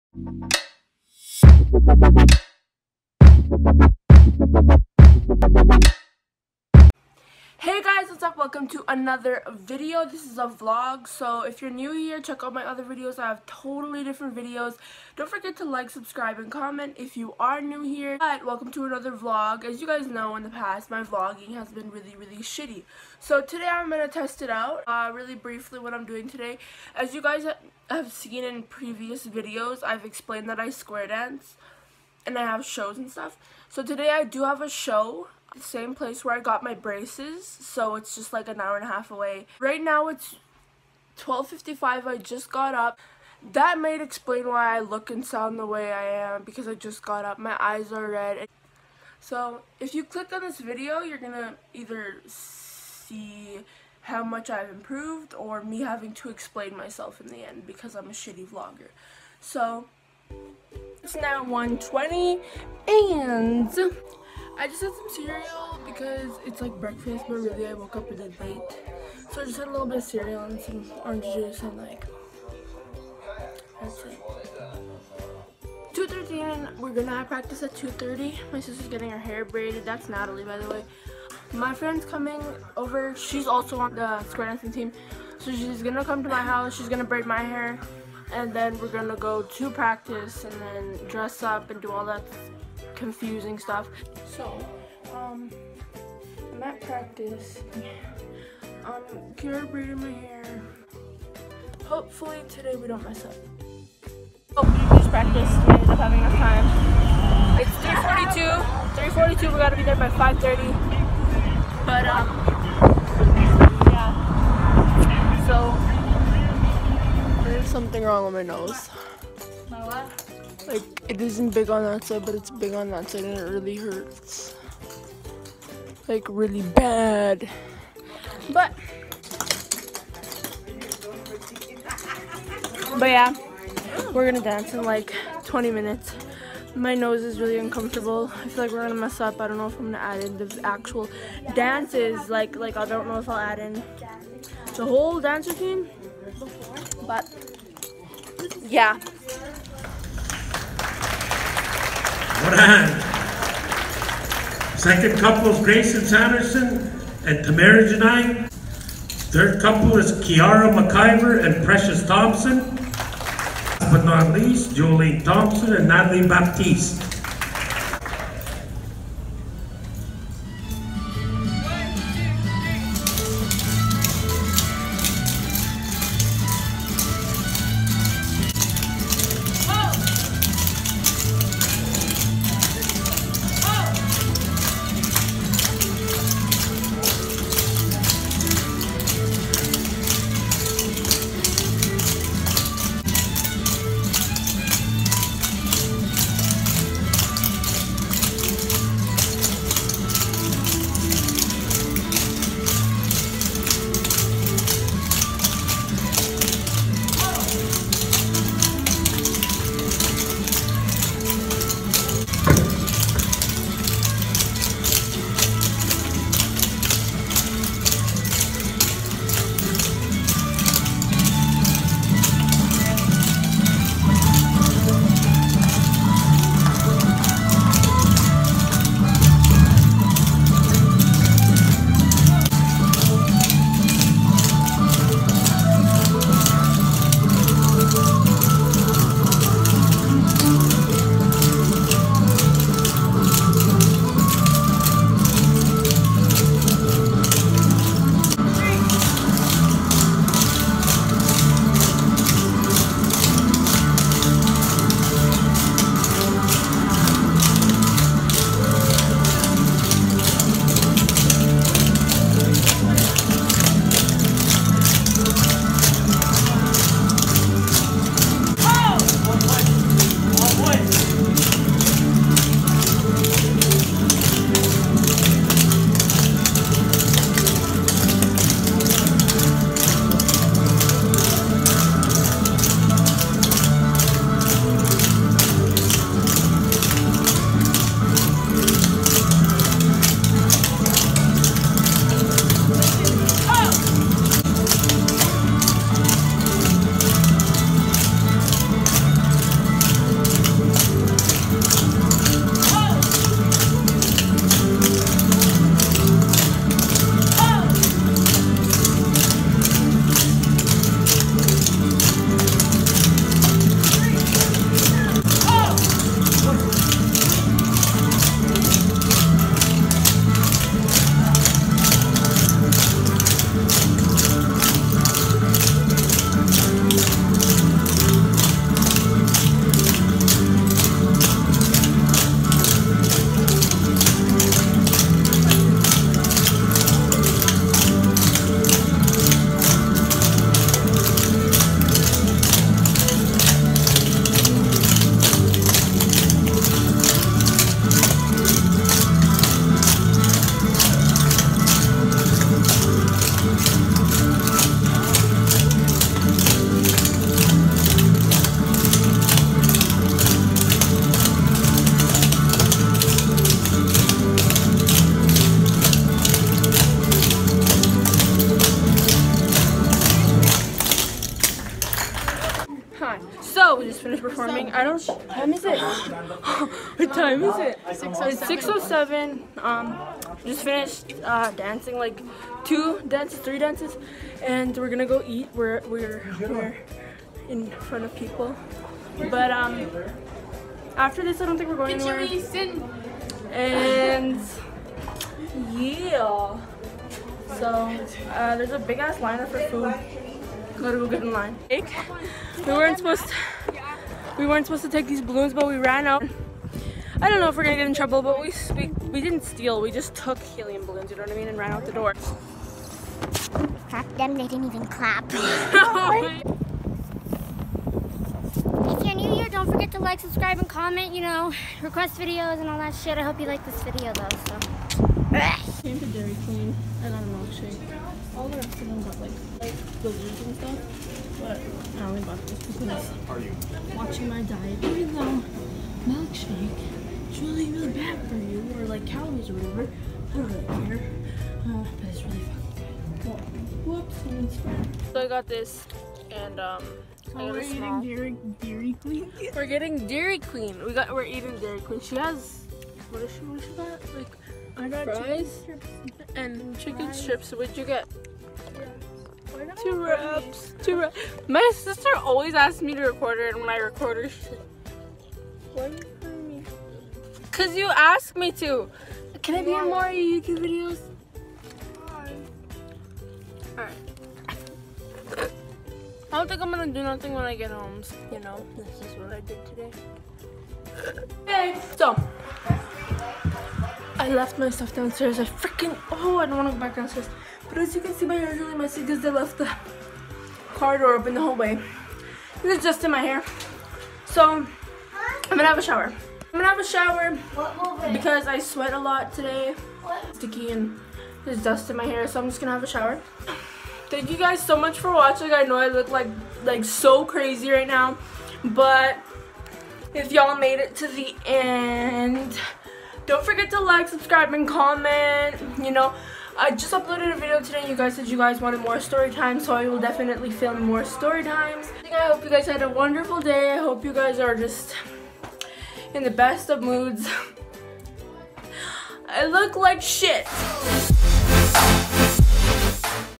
Square dancing vlog!
I think my vlogging skills have improved even if it was the slightest lolll , hope you enjoyed seeing some of my square dancing thou I couldn’t put the full routines in.